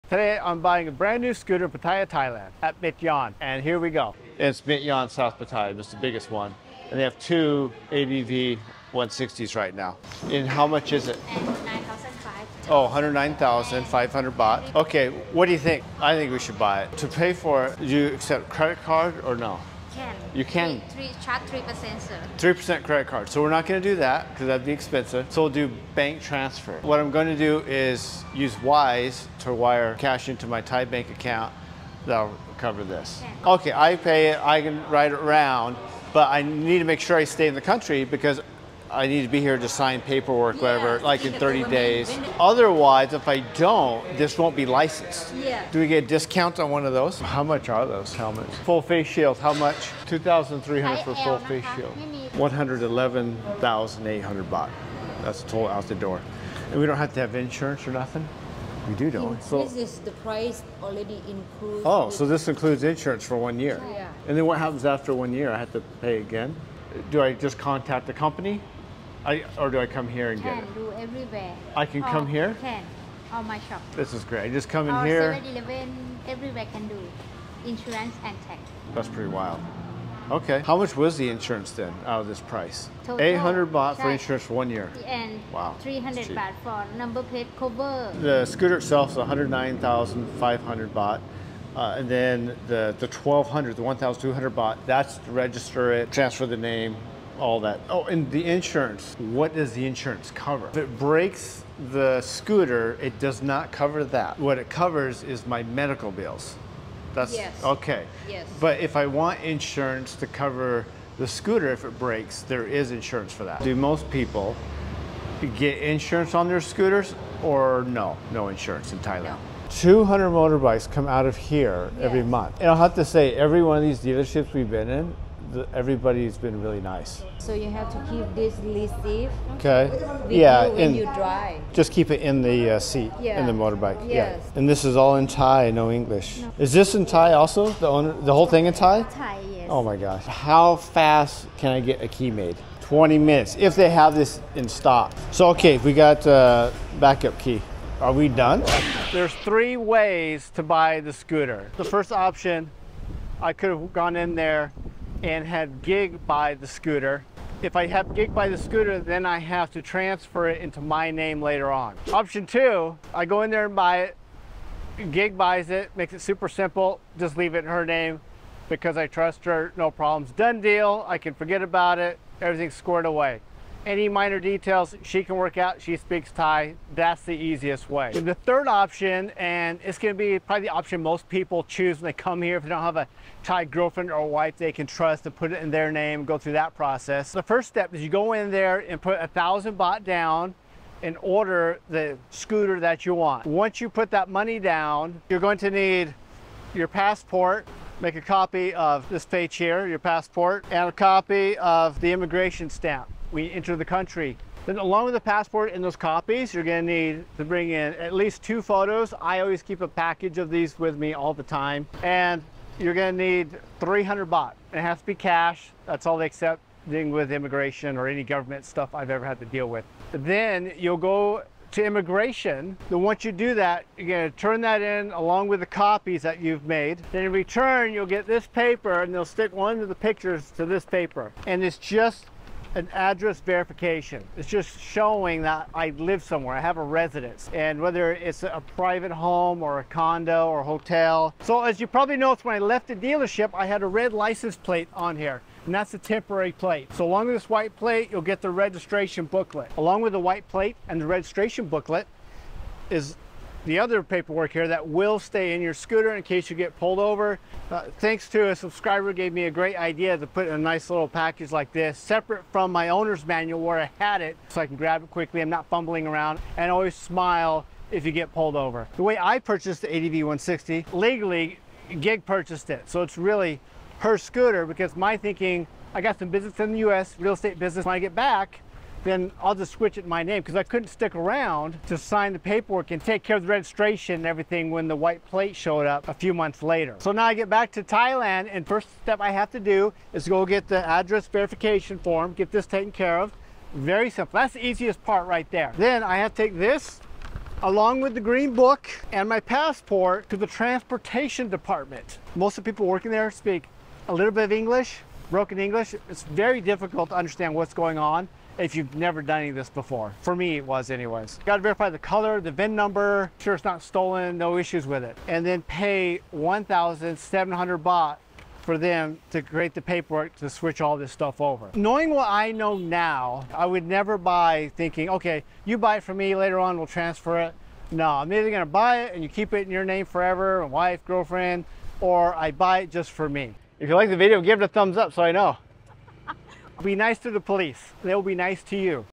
How To Buy A Scooter or Motorcycle In Thailand
I share my buying experience purchasing a new Honda ADV 160 from one of many Mityon dealerships in Pattaya, Thailand.   The process is very simple.  The most challenging part is the language barrier, which takes some patience and asking the same questions multiple times.  Once I had the Residence Certificate, the buying process at Mityon only took 30 minutes.